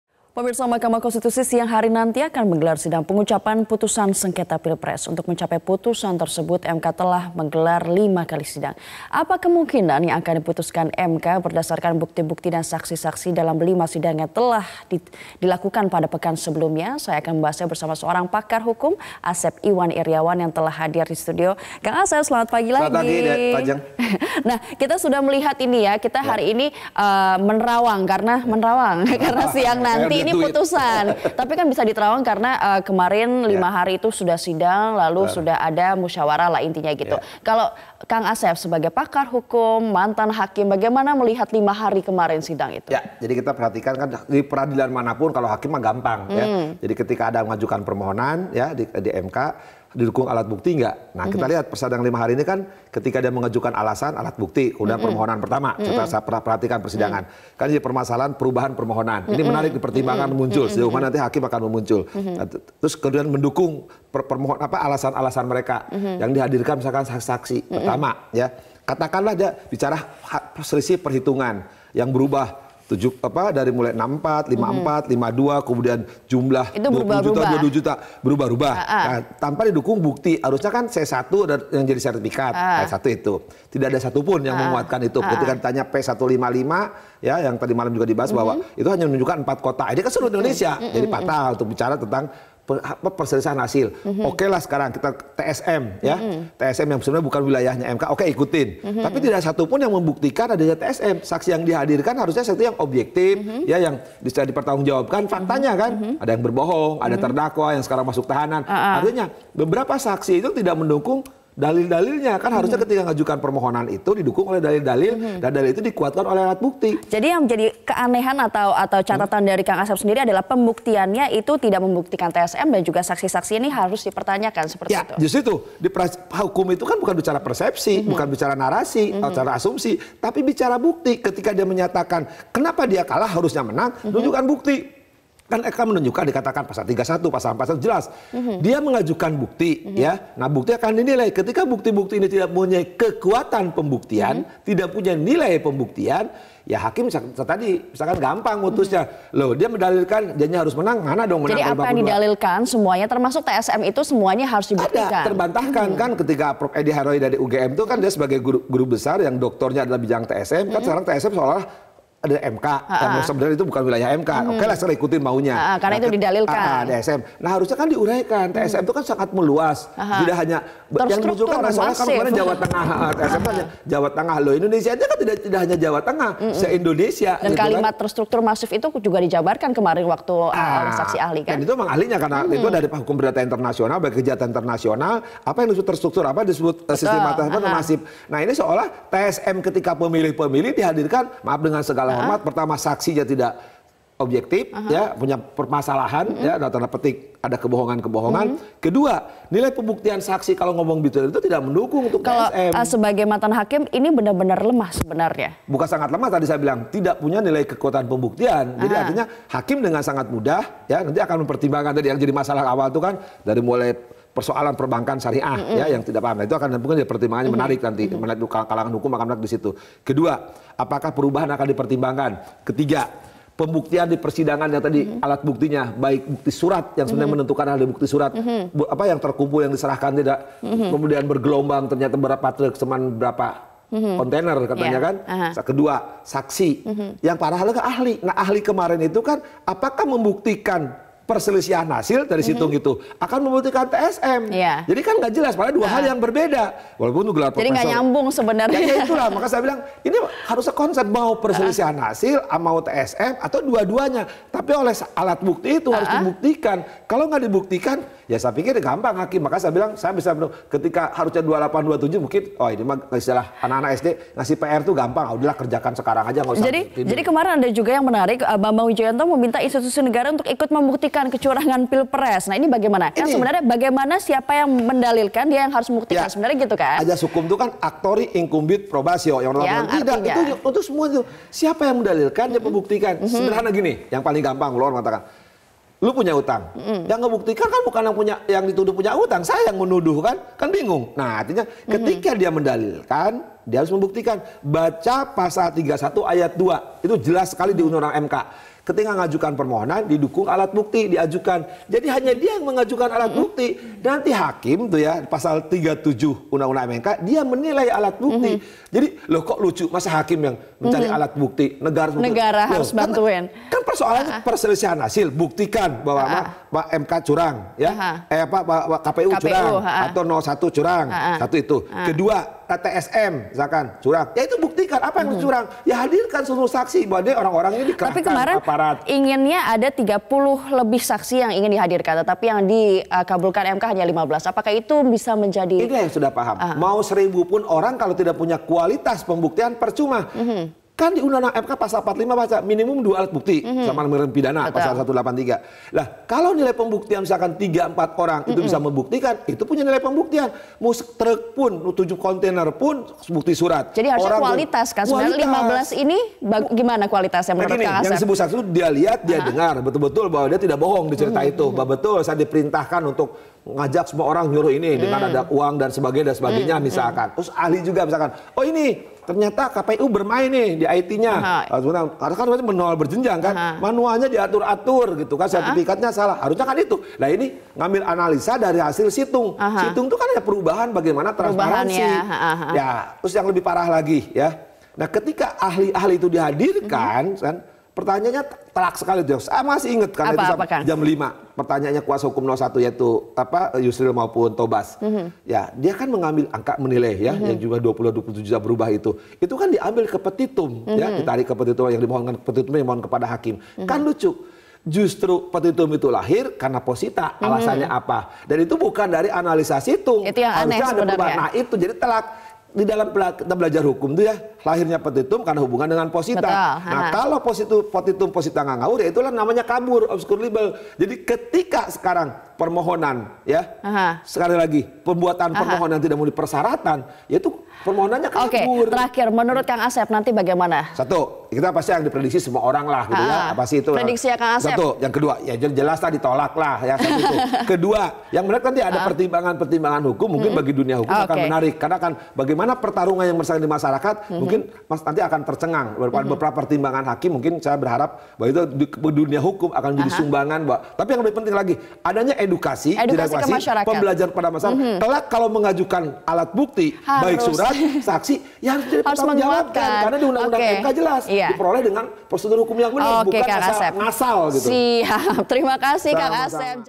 Lemah! Pakar Hukum Pesimis Gugatan Prabowo Dikabulkan MK
Mahkamah Konstitusi akan menggelar sidang pembacaan putusan sengketa Pilpres pada Kamis (27/06/2019). Pakar hukum Asep Iwan Iriawan menilai dalil pemohon mengenai kecurangan secara terstruktur, sistematis dan masif (TSM) tidak didukung bukti yang kuat dalam persidangan. Bukti pihak pemohon belum bisa membuktikan adanya kecurangan TSM.